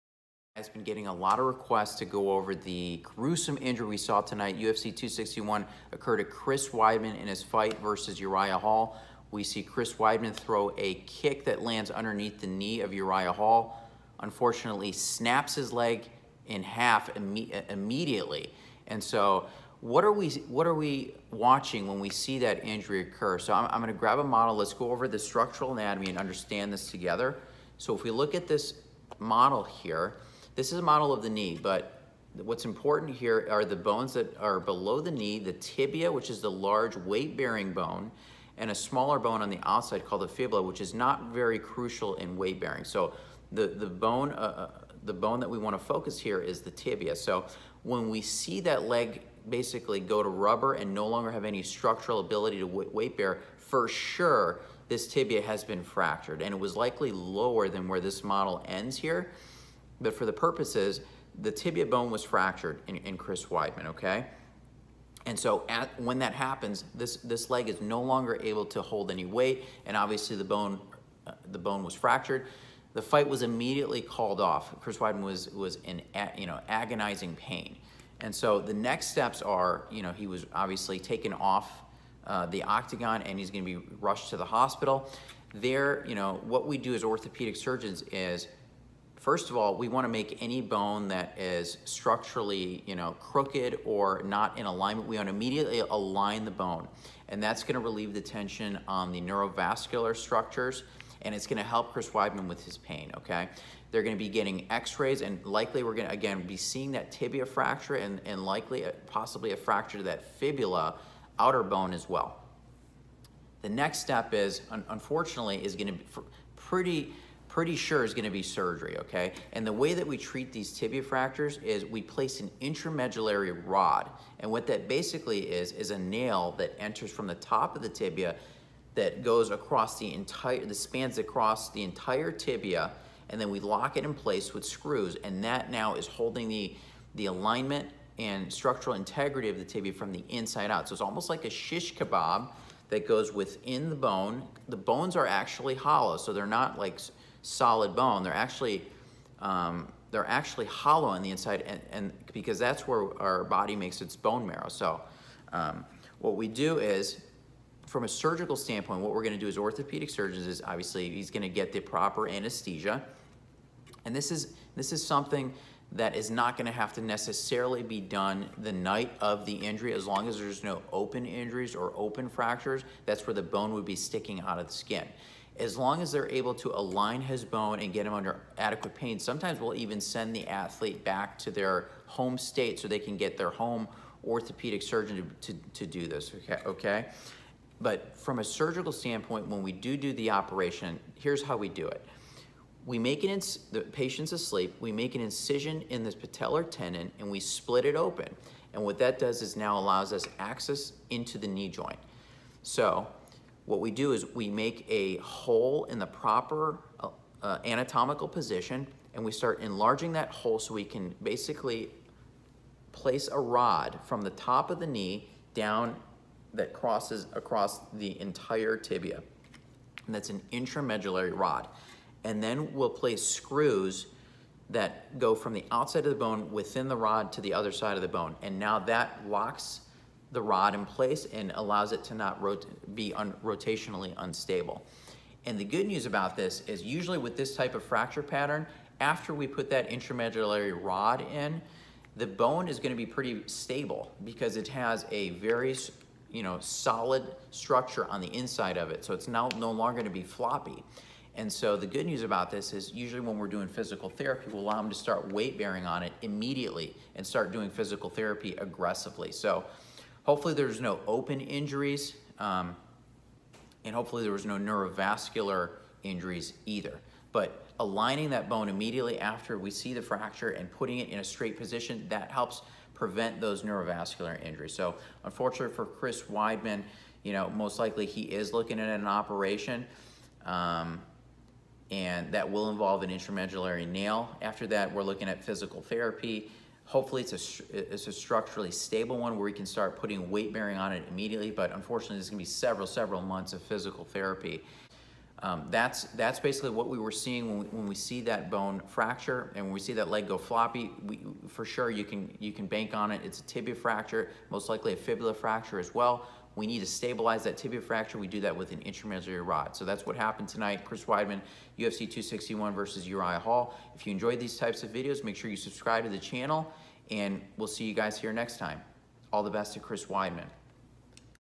Chris Weidman's LEG SNAPPING IN HALF Explained by MMA Ringside Orthopedic Surgeon!
My Medical Breakdown on Chris Weidman‘s severe leg injury at UFC 261. 😷

Leave your questions and comments below and let me know if this Medical Breakdown is helpful by making this injury easier to understand!